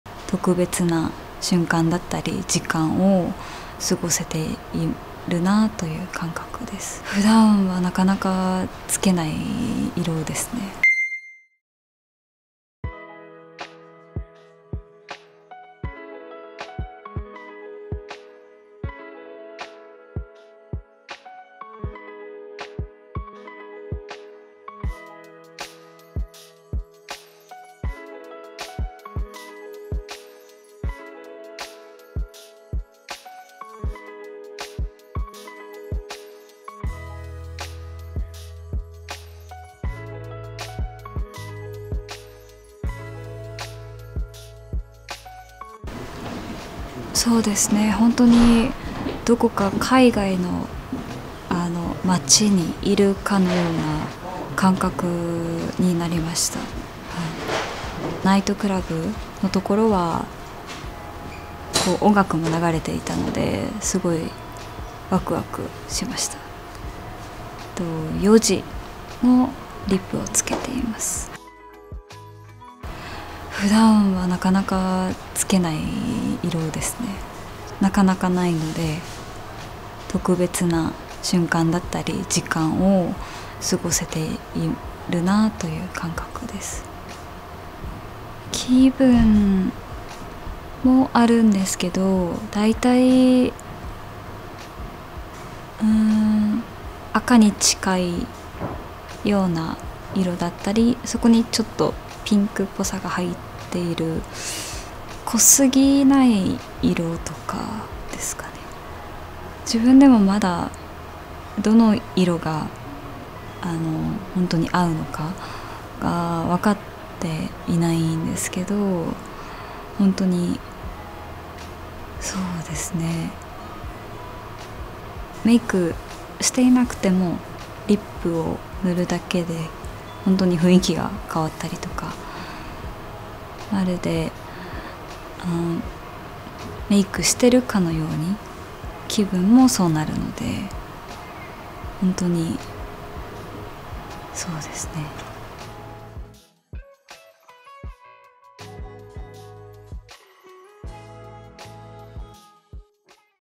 平手友梨奈、春らしいスタイルで登場！普段のメイクについても明かす シャネル「ルージュ アリュール ヴェルヴェット ニュイ ブランシュ／眠らない夜」プレビュー
俳優の橋本愛、平手友梨奈がシャネル「ルージュ アリュール ヴェルヴェット ニュイ ブランシュ／眠らない夜」プレビューに登場した。

🎬平手友梨奈、“カルティエ”コーデでクールに決める！　「TIME UNLIMITED - カルティエ ウォッチ 時を超える」オープニングレセプション